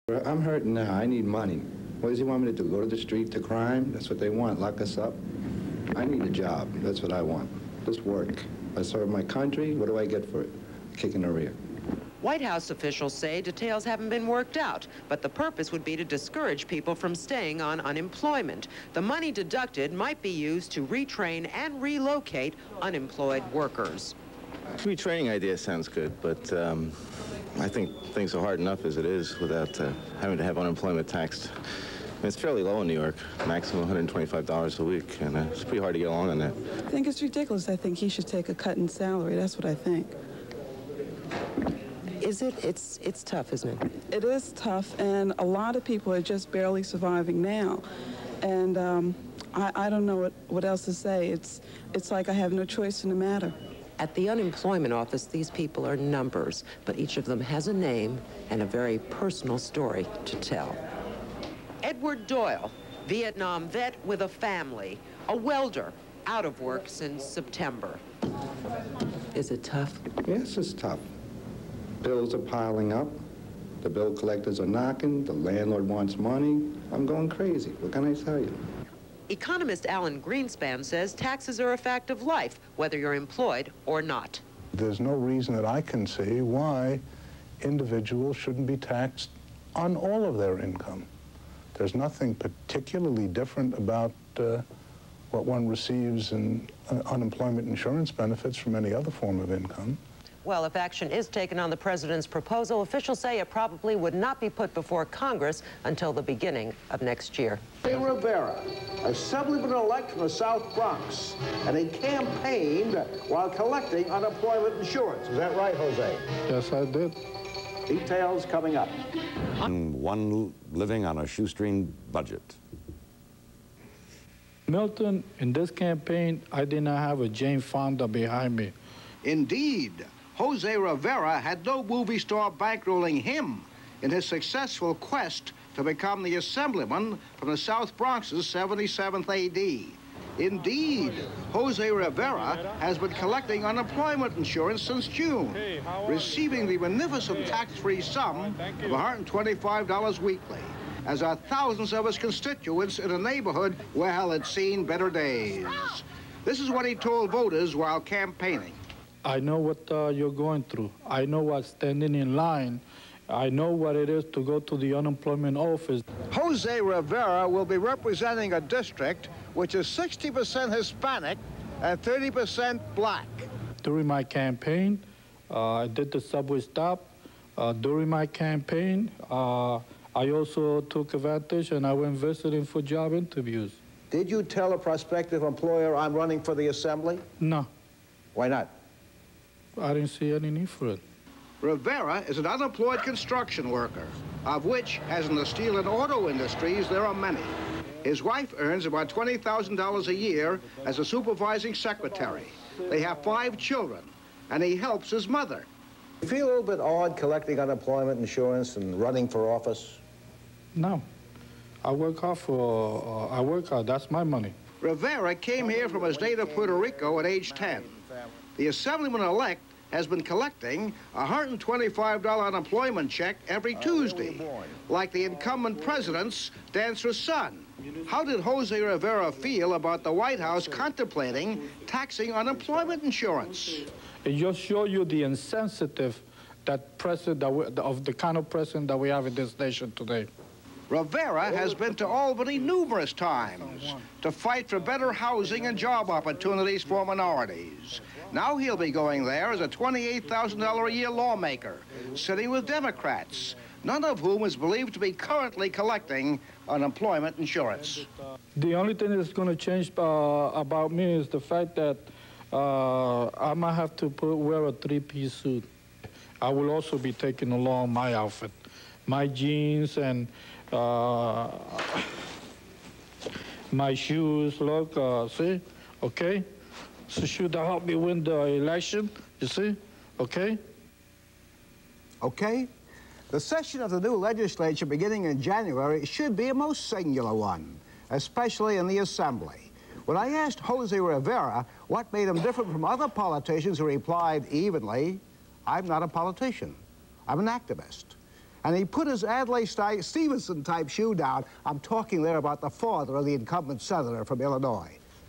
José Rivera on Channel 7.mp4
Jose Rivera running for the 77th Assembly District.Have You Ever Been Unemploy, had to Stand in a Line ? If You Are Unemploy 'Then Do What I did, Run for Office" 1982 - and I Still Have My Shoes _